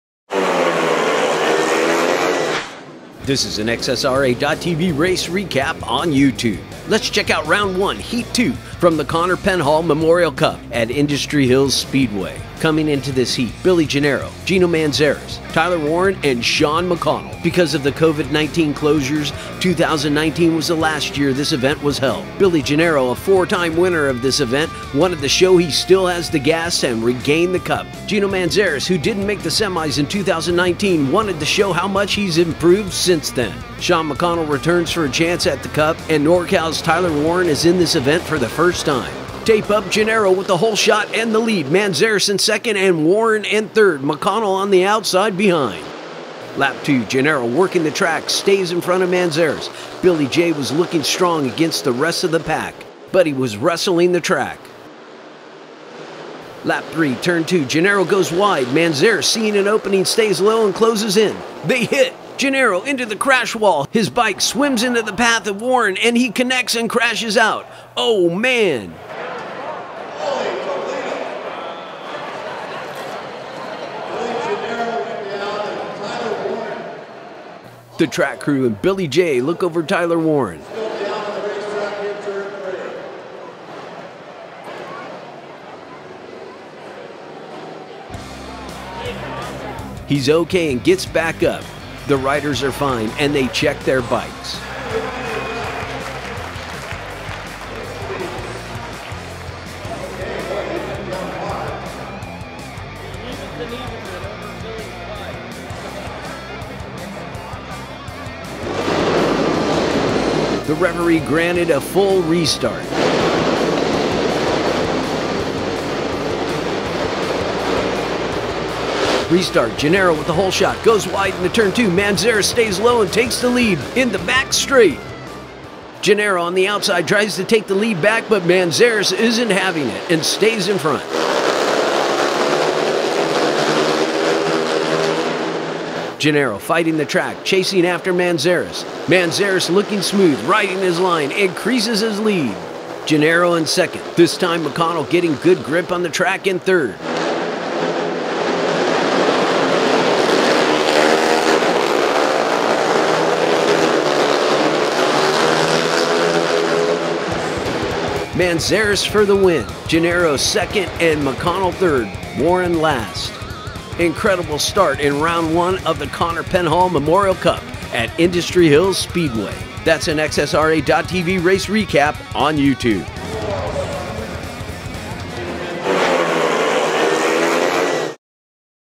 Round 1 - Heat 2 at the Connor Penhall Memorial Cup Speedway 2022
Watch Heat 2 from the 2022 Connor Penhall Memorial Cup race at Industry Hills Speedway. This was the return for the Memorial Cup event after the Covid-19 closures around the world.